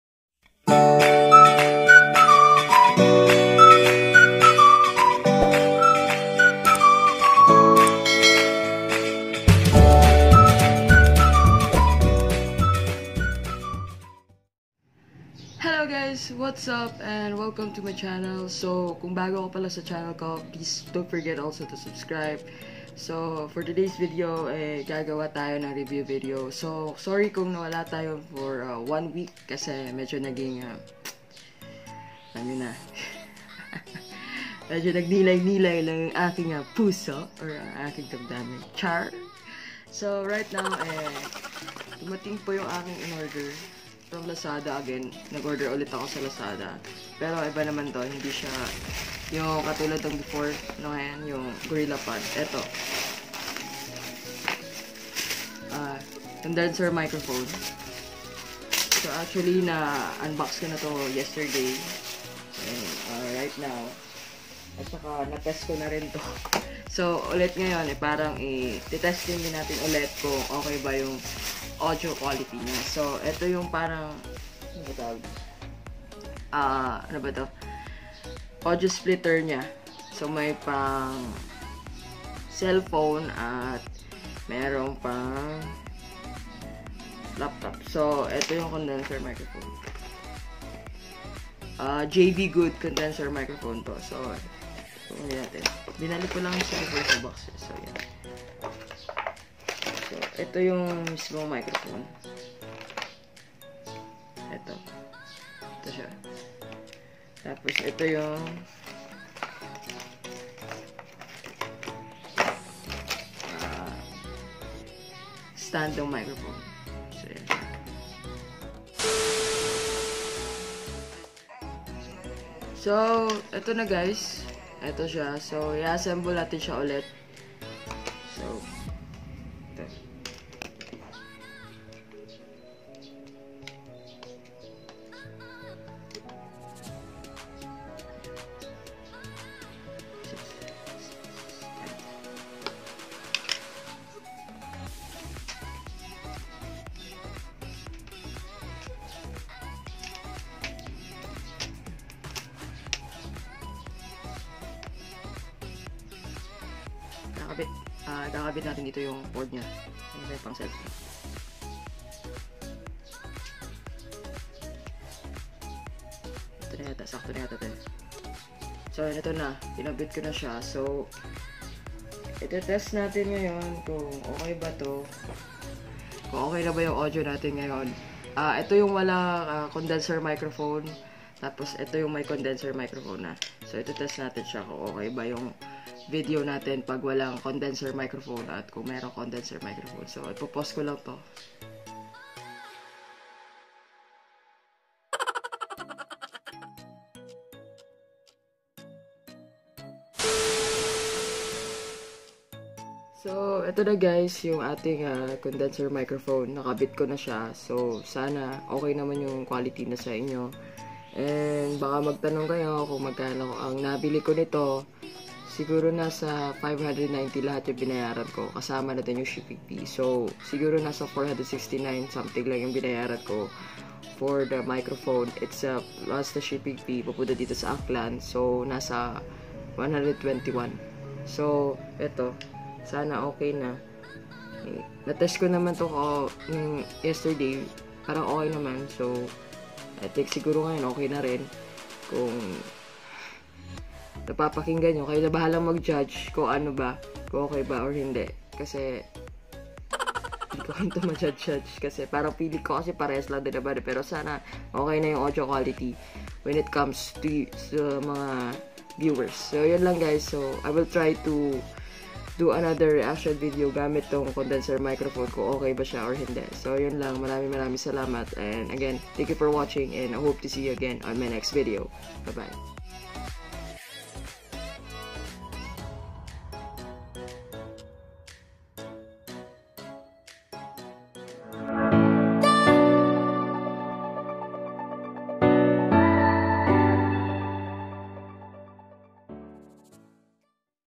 Review of JVGood Condenser Microphone from Lazada
This is my review video for JVGood Condenser Microphone I bought from Lazada. Share your thoughts about this product on the comment section.

Based on my observation this mic is good for livestreaming, audio recording.

and see for yourself for the quality.

PRICE:
• JVGood Mic = P469
• Shipping fee (Aklan) = P121

Hi! My name is Angelli from Philippines.

I do random reaction videos and vlogs.